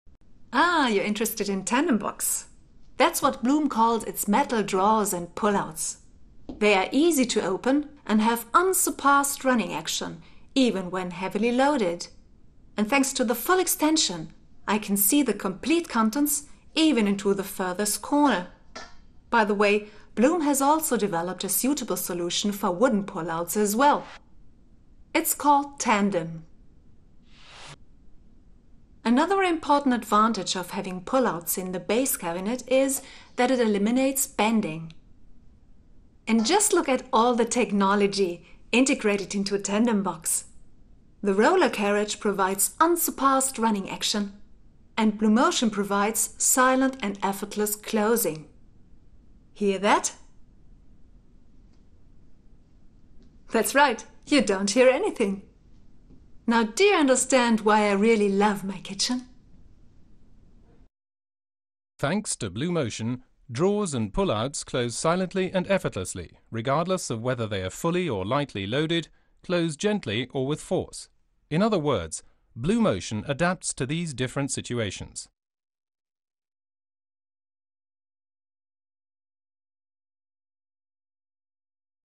blum TANDEMBOX
blum TANDEMBOX: TANDEMBOX is a versatile system for constructing drawers, inner drawers, high fronted pull-outs and inner pull-outs of the highest quality. With the integrated BLUMOTION, the system closes silently and effortlessly.

For improved organisation and overview, TANDEMBOX can also be equipped with ORGA-LINE inner dividing systems.